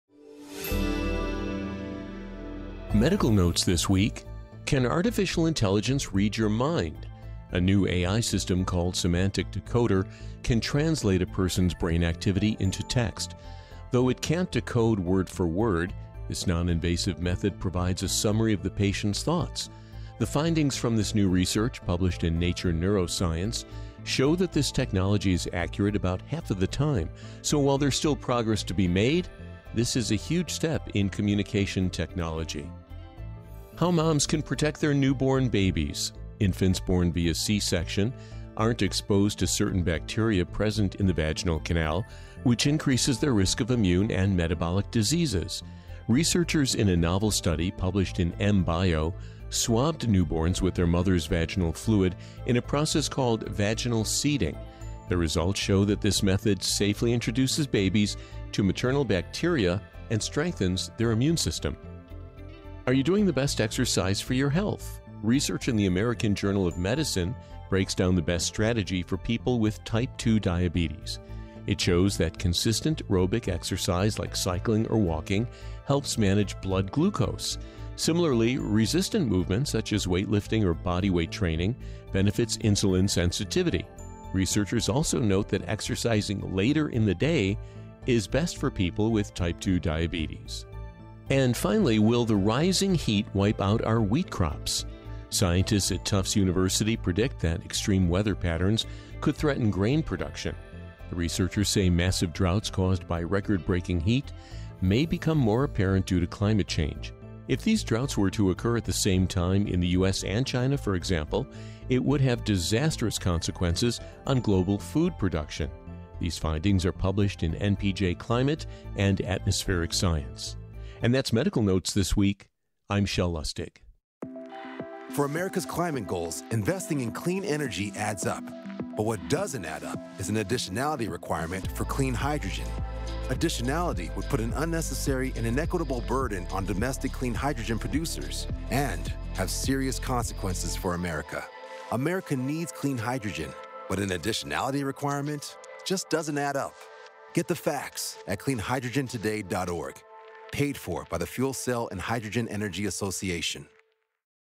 Medical Notes: How AI Can Read Your Mind, Protecting Your Newborn Baby, And If Extreme Weather...
Can artificial intelligence read your mind? Then, How moms can protect their newborn babies. Then, are you doing the best exercise for your health? And finally… will the rising heat wipe out our wheat crops?
Learn more about your ad choices.